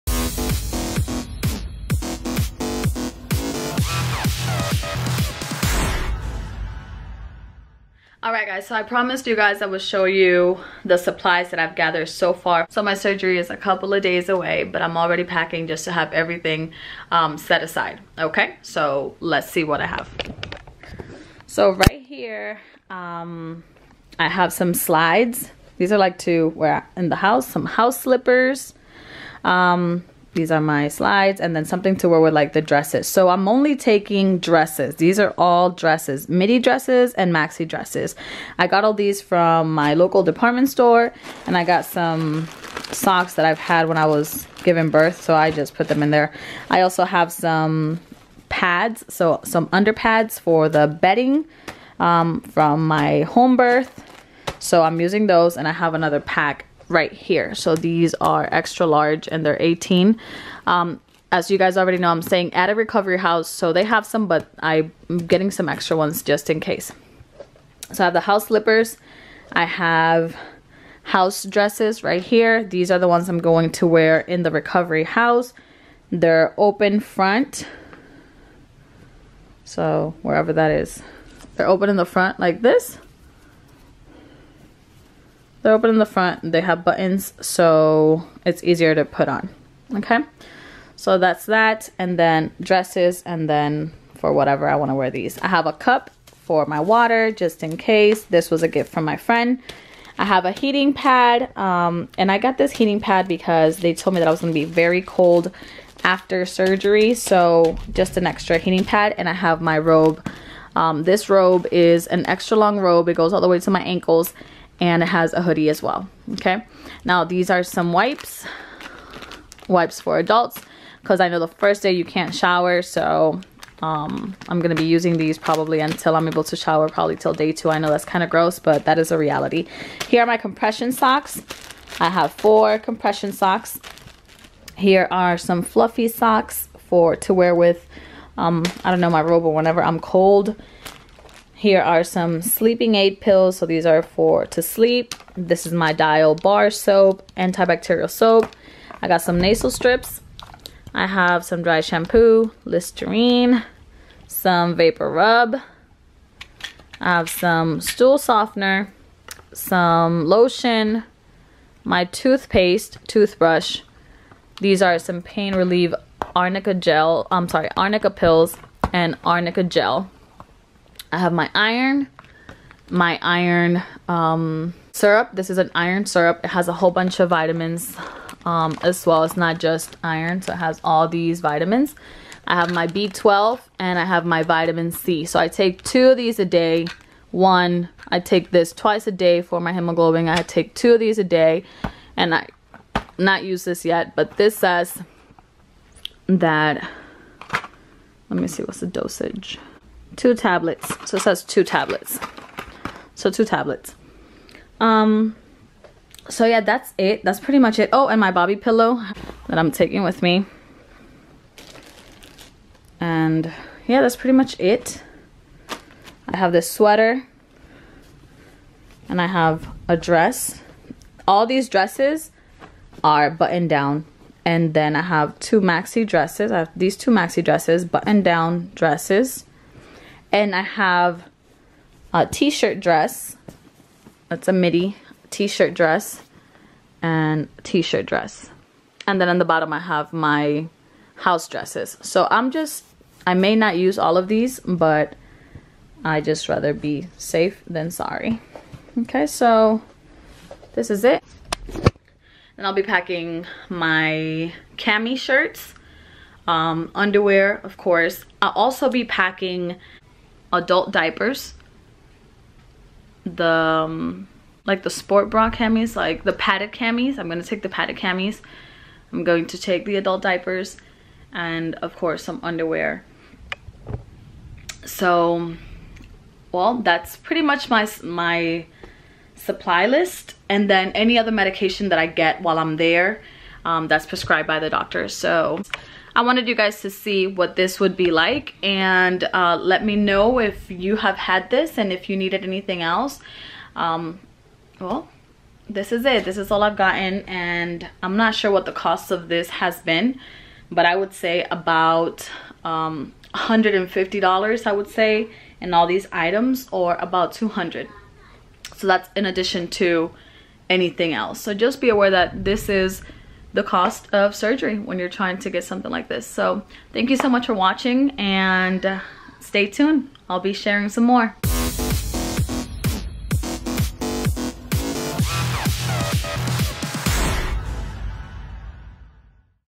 What to Pack BBL and Tummy Tuck @Avana Plastic Surgery
Here is a list of items and things that i found most useful or that you may want to considered.

Must Have
1- Robe- NY Threads Women Fleece Hooded Bathrobe - Plush Long Robe (Medium, Taupe)
2- Arnica Gel-  Boiron Arnicare Gel 4.1 Ounce (Pack of 1) Topical Pain Relief Gel
3- Arnica Pills- Boiron Arnicare, 60 Tablets, Homeopathic Medicine for Pain Relief
4- Arnica Tea
5- Baby Wipes
6- Hibiclens Liq 8 Oz
7- Compression Socks - Open toe
8- Half Camis- 4 Pack Women's Seamless Wireless Half Cami Unpadded Bra Tops
9- Warm Fluffy Socks
10- Multi Purpose Slides
11- Comfortable Dresses
12- Lipo Foams- Impresa 3 Pack Lipo Foam -
13- Thin Lipo Foams- M&D 0001 Lipo Side Foam Lateral Protectors Liposuction Boards
14- Full Back Molder- M&D Colombian Shape wear Post surgery Lipo Lumbar Molder Back Board
15- Ab Board- M&D 0105 Compression Flattening Ab Lipo Board
16- Small Lower Back Molder- M&D 9016 Lipo Foam Lumbar Molder
17- Extra Compression Garment
18- Dial Antibacterial Soap
19- Toiletries
20- Pads in case of Period
21- Underwear
22- Stool Softener
23- Tylenol Extra Strength
24- Tylenol PM
25- Protein Shakes
26-Gatorade or Pedialyte